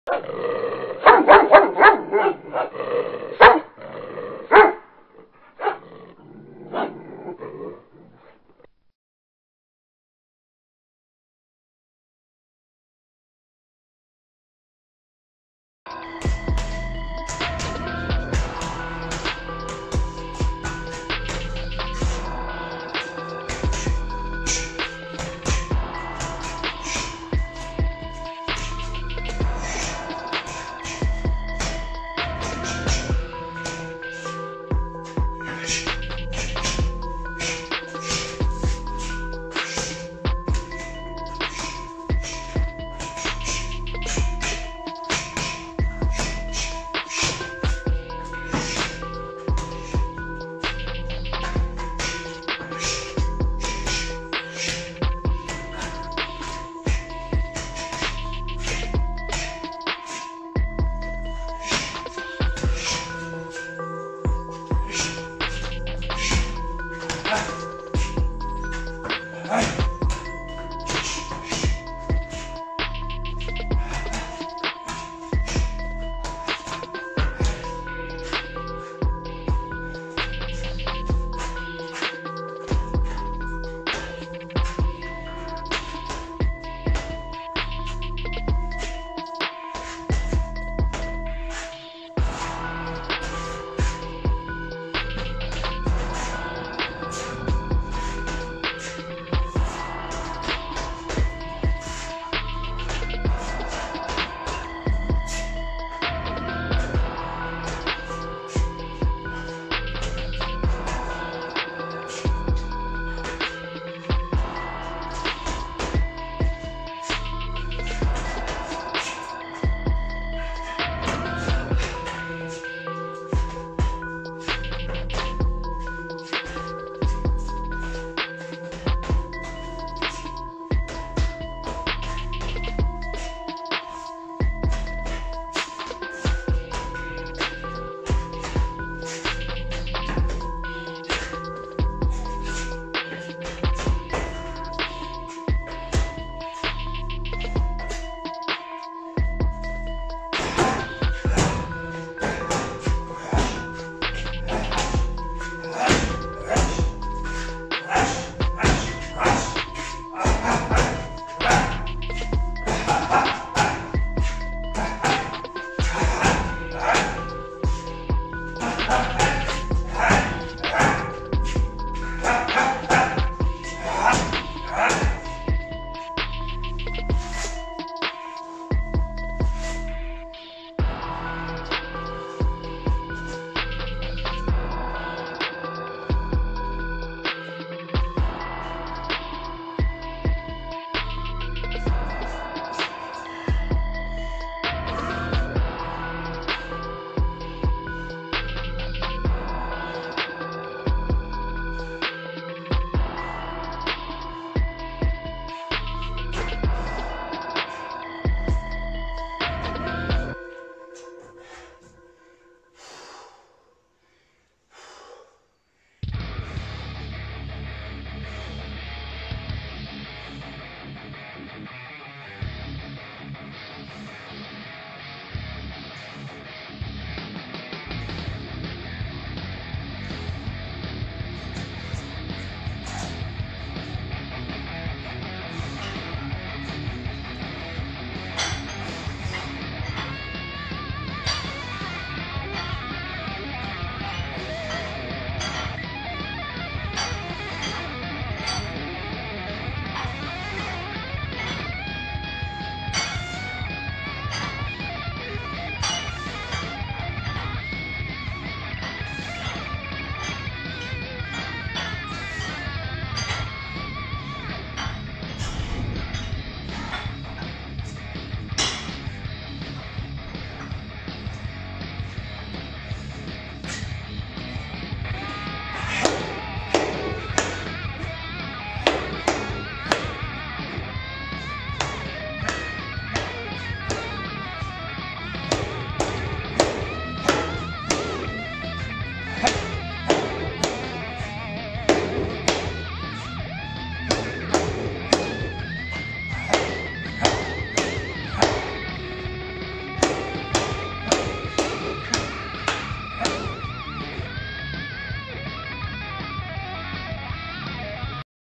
Home workout. Cirquitje.
Zelfgemaakte "double end bag" toegevoegd aan m'n garage set up. Met een 't shirt, een bal en wat touwtjes. Als je er ook eentje wil maken dan kun je op youtube een paar voorbeelden vinden.
Het cirquitje bestond uit :
1 rammen met de stok tegen de autobanden, als je geen autobandjes kunt ophangen kun je het in schaduw boks vorm doen.
2 boksen tegen de kleine bal
3 double end bag, precisie werk en bewegen
4 Power shots tegen de muur met kussens
5 push ups met handen op 2 stenen. Lekker diep doorzakken
6 rows voor de rug met home made suspension trainer. Tutorials ook te vinden op YouTube
7 curls en shoulder press met de dumbells.
Vooraf warming up met touwtje springen en wat extra focus op stretching.
Tussendoor wat hill sprints gedaan en met wat honden gelopen en getraind.

Stay safe. Stay fit.
Kida